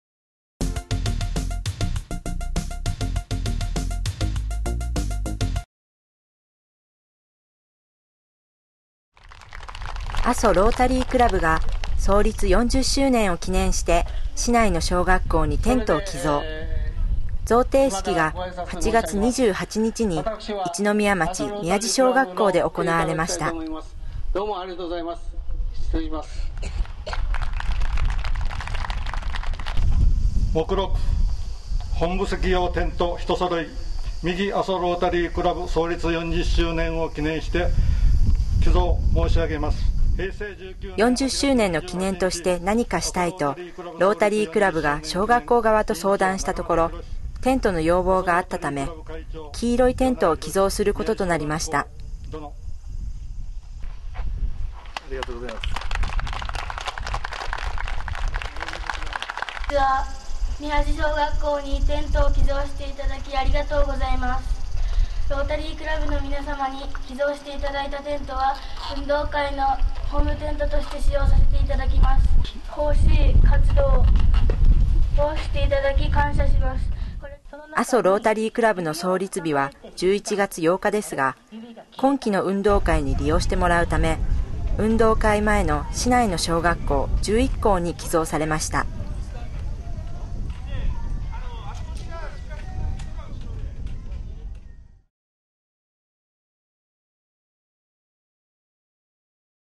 平成19年阿蘇ロータリークラブがテント寄贈
阿蘇ロータリークラブが創立40周年を記念して市内の小学校にテントを寄贈。